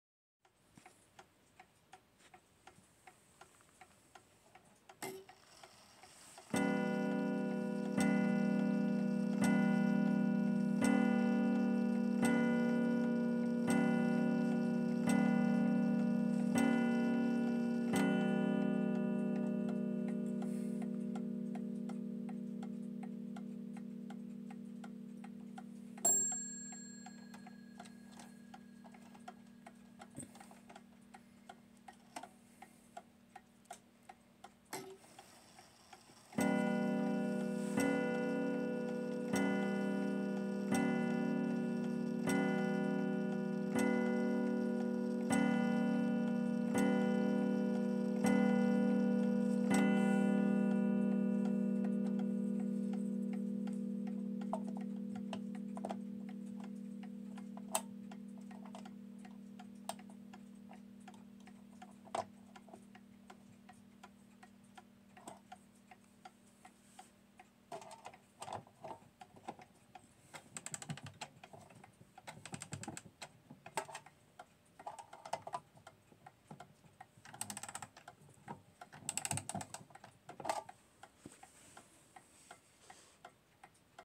Fully Restored Sessions time and strike black mantel clock, cathedral gong and bell
This is a fully restored Sessions time and strike black mantel clock. Strikes hours with a rich cathedral gong sound. Half hours on a crisp sounding bell. Comes with key and pendulum, will be packed well for shipping.
To purchase this beautiful clock, please visit pbclocks.etsy.com